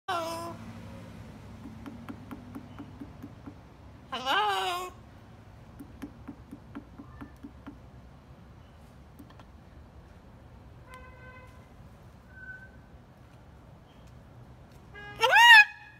Cody the Macaw talking to some Sandhill Cranes.
Cody hopes you enjoy today's video and have a great day!